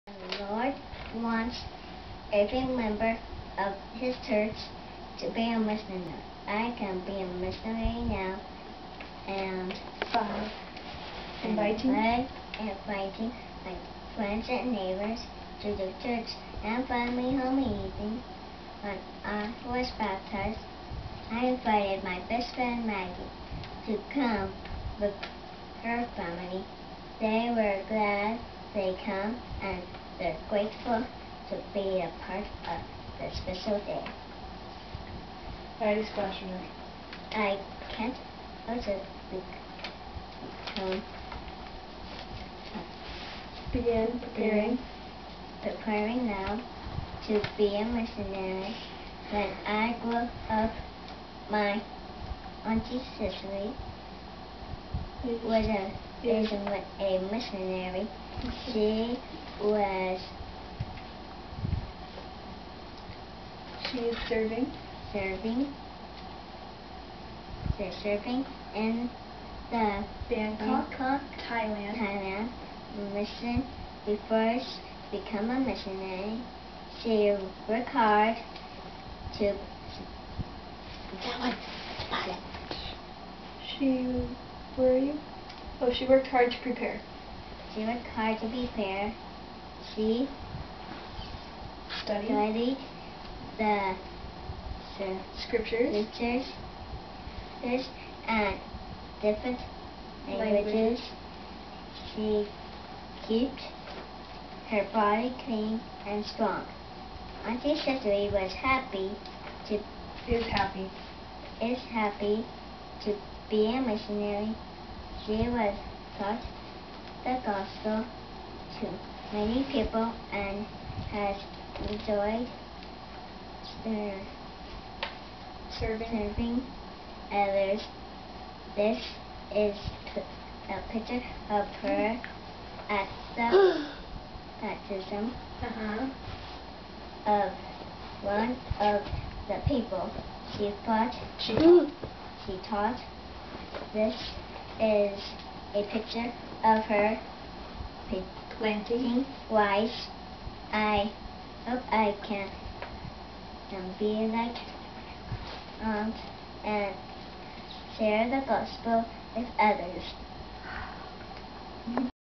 Dakotah's talk on being a missionary
with a little help from big sister Dakotah gives us a preveiw of her talk for Primary